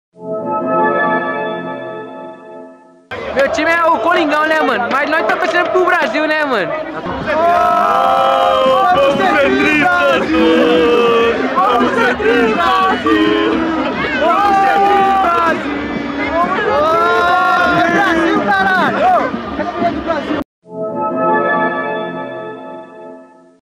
Corintiano torce para o rival Santos no Mundial
O Sanros está enfrentando o Barceloma no Japão